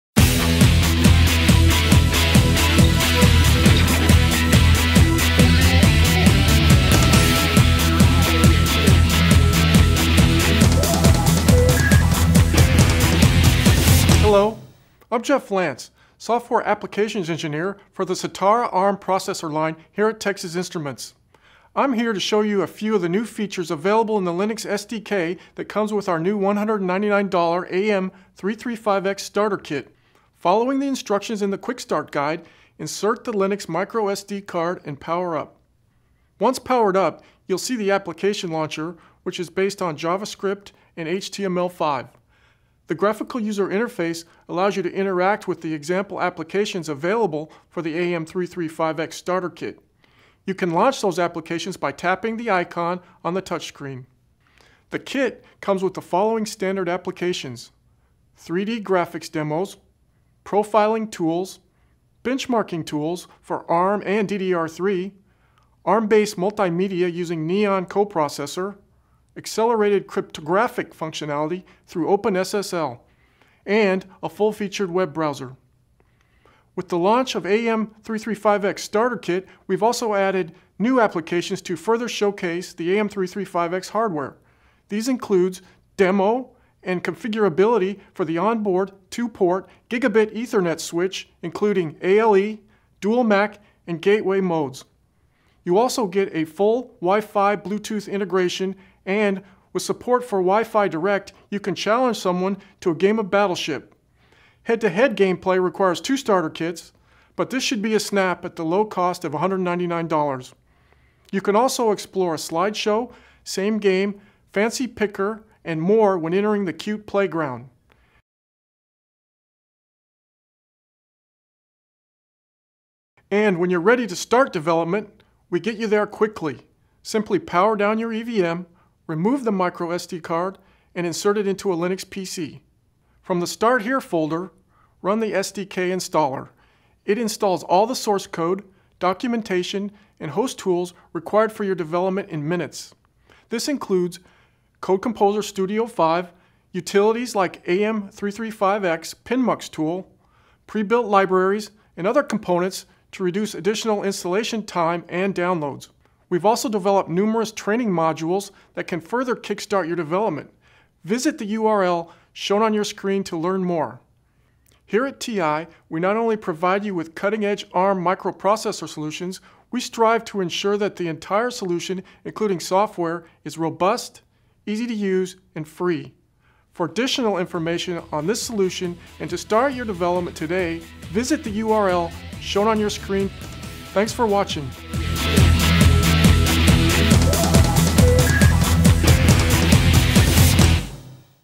Starting Linux development on TI Sitara AM335x Starter Kit
Considering Linux developement on the Sitara AM335x ARM processor?  See how simple it is to get started on the new $199 Starter Kit.  Explore the initial steps that will have you developing in no time.